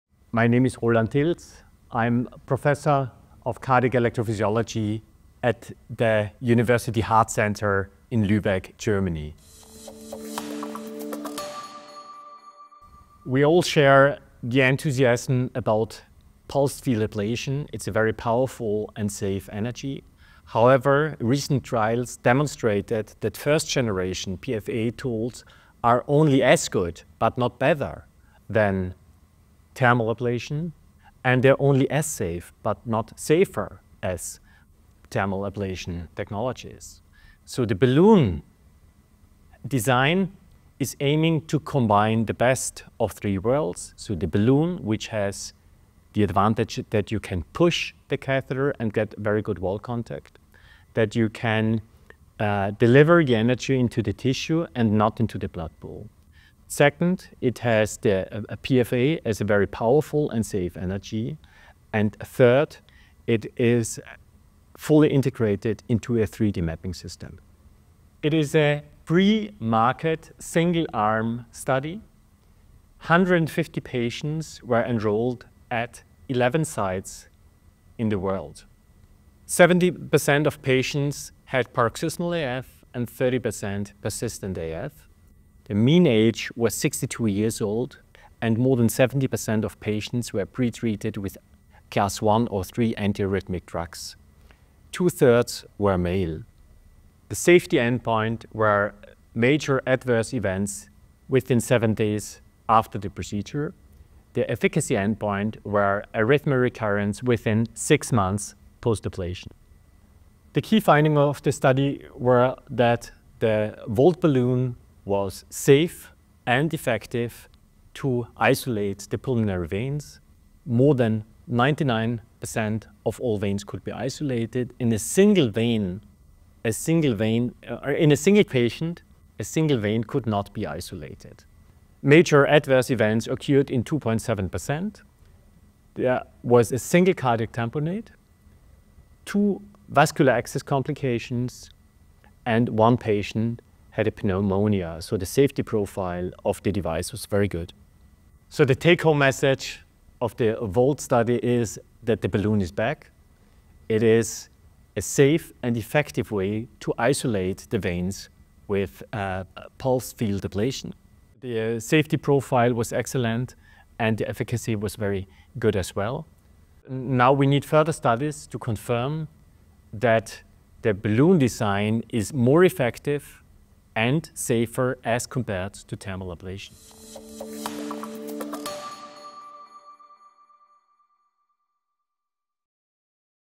AF Symposium 25: Balloon-Based PFA for De Novo PVI in PAF and PersAF
AF Symposium 25 - Safety and effectiveness results show the Volt PFA system (pulsed field ablation; Abbott) for PVI (pulmonary vein isolation) is a successful treatment option for paroxysmal and persistent atrial fibrillation patients.

Prof Roland Tilz (University Heart Centre Lübeck, Lübeck, DE) joins us onsite at AF Symposium 25 to discuss the findings of balloon-based PFA for PVI. The pre-market, single-arm study's safety endpoint was major adverse events within 7 days post-ablation, and the efficacy endpoint was arrhythmia recurrence within 6 months post-ablation.

Findings showed that the Volt PFA system met its safety and efficacy endpoints with a low occurrence of major adverse events.

Interview Questions:
1. What is the reasoning behind the trial?
2. What was the study design, safety endpoints and patient population?
3. What are the key findings?
4. What are the take-home messages for practice?
5. What further study is needed?

Recorded on-site at AF Symposium in Boston, 2025.

Editors: Yazmin Sadik, Jordan Rance.
Videographers: Oliver Miles, David Ben-Harosh.
Support: This is an independent interview produced by Arrhythmia Academy.